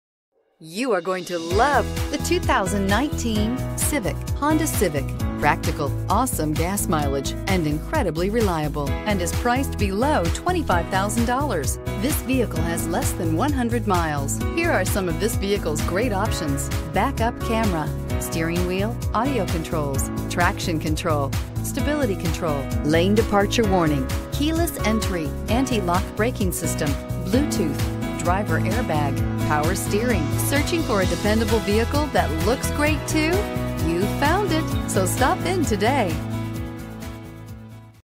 2019 Honda Civic Sedan 38410
2019 HONDA CIVIC SEDAN Mesa, AZ
Stock# 38410

6229 E Auto Park Dr

You'll love this 2019 Honda Civic Sedan. This is a car you'll want to take home. With 8 miles, it features Variable transmission and an exterior color of MODERN STEEL ME. Call us and be the first to open the car door today!